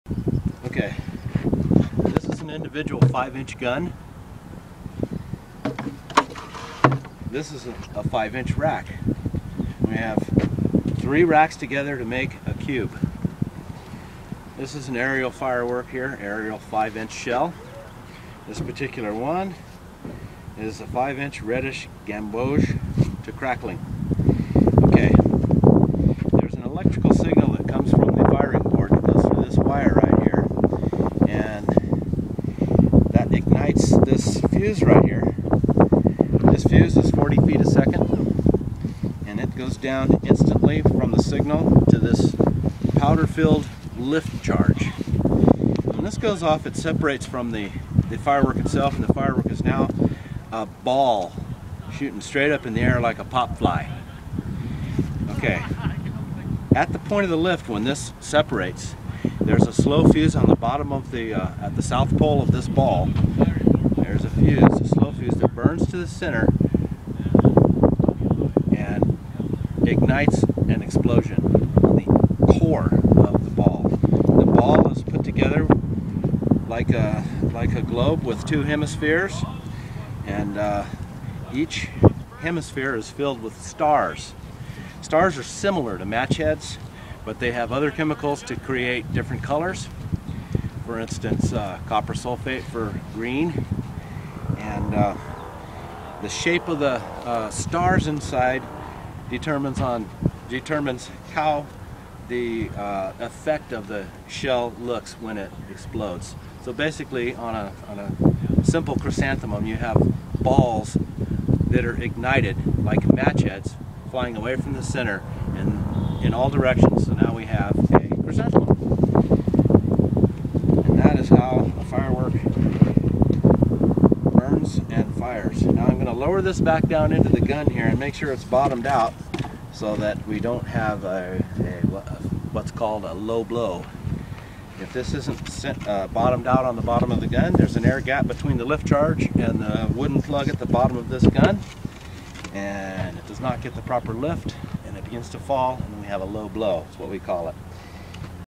How fireworks work
Mike Bush, a member of the Pyro Spectaculars crew preparing for the July 4, 2012 fireworks show at University of Redlands, explains how a fireworks shell launches and explodes in the sky.